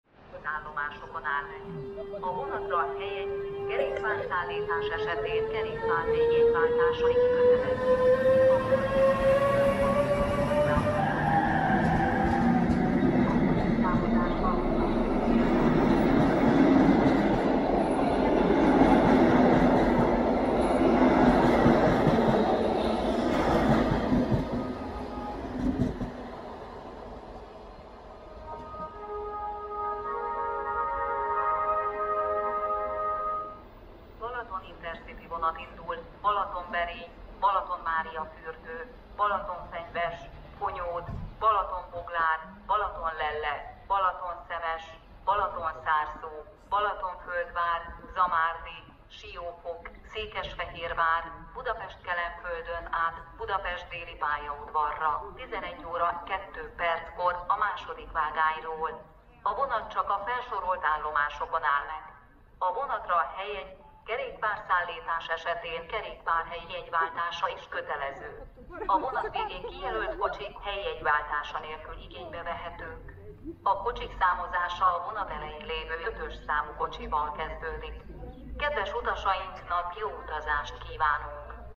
H-START 480 004 indul a Balaton IC-vel Balatonszentgyörgyről, 2021. 10. 16. [4K]
H-START 480 004 ("170 éves a magyar vasút" TRAXX) indul a 862 számú Budapest-Déli pu. - Keszthely Balaton IC-vel Balatonszentgyörgyről, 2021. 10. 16-án.
H-START 480 004 (Werbelok "170 years the hungarian railway") departs from Balatonszentgyörgy, with number 862 Budapest-Déli Station - Keszthely Balaton InterCity-train, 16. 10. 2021.
Video.: Petrás Bence